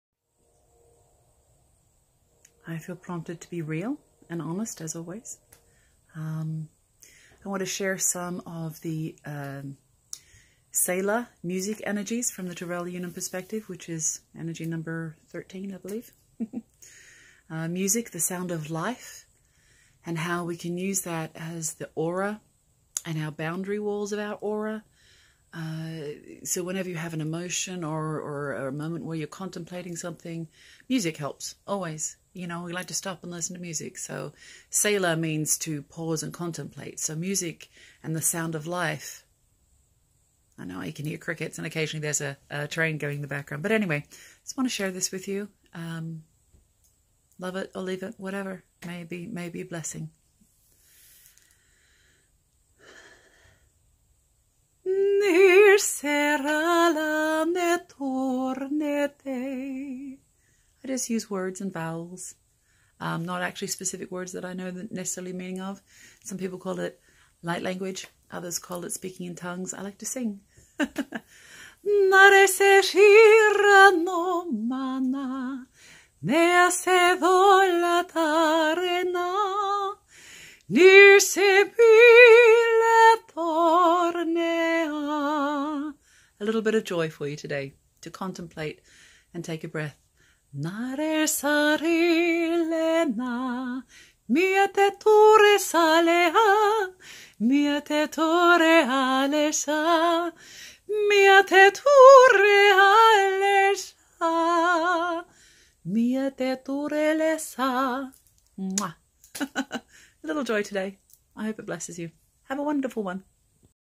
Light Language (tounges) Song of Joy.
TR3V3L3UNUM perspective philosophy Energy #13 is Music/Sound of Life with the Positive Aspect of Selah.
Selah means to pause & contemplate. Music often aids our contemplation of life. Join me for a couple minutes as I contemplate Joy through Light Language Song; (the Vocalization of energy & emotion through seeming random sound.)